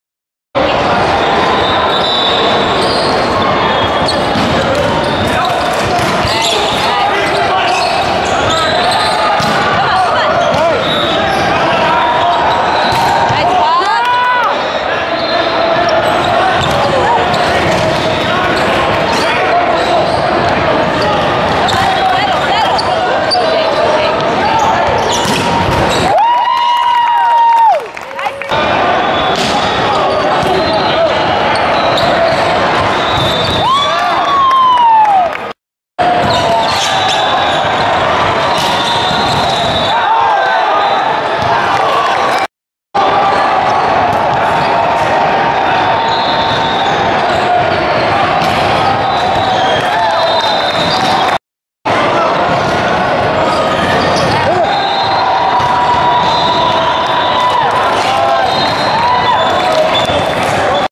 949 17 vs evolution 18 game 3 bronze holiday classic
2014 SCVA Boys Holiday Classic Volleyball Tournament
Bronze Bracket Championship Match
Game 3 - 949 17 vs Evolution 18